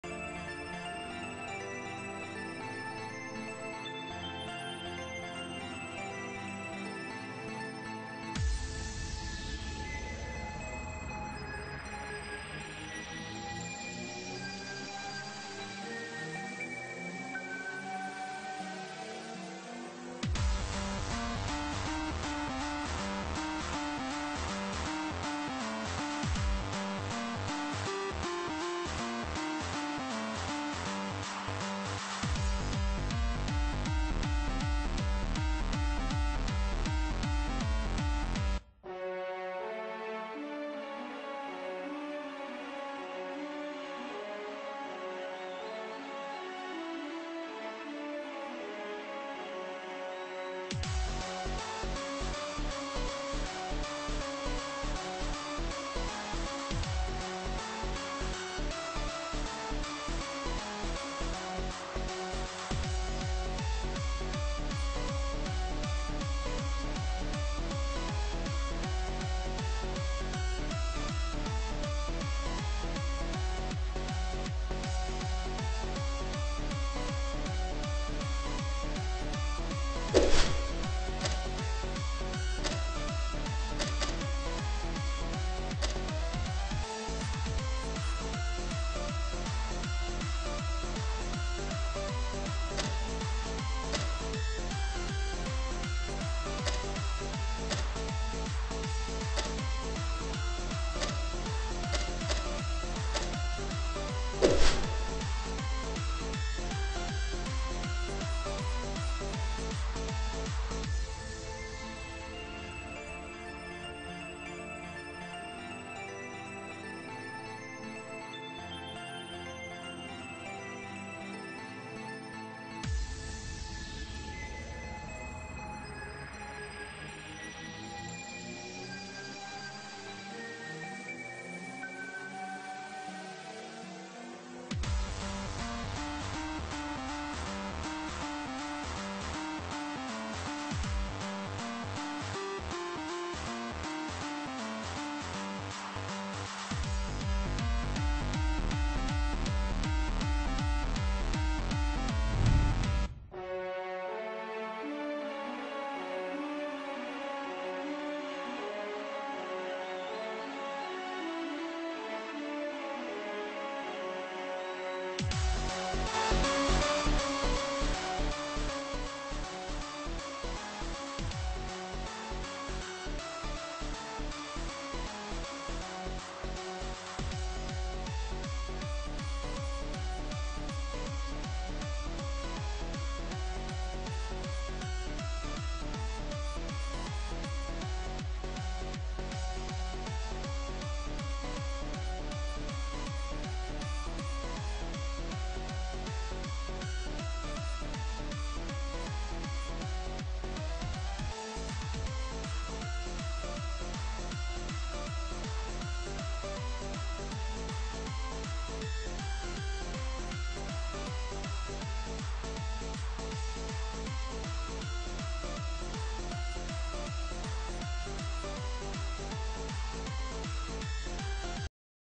when hackers expose themselves [Bym]
the user 'AlphaProOmega' was 'testing' scripts that played some menu sound effects, changed time of day, and deleted some of the terrain, he fully exposed himself when i noticed he had the npc chat, which he then admitted to hacking.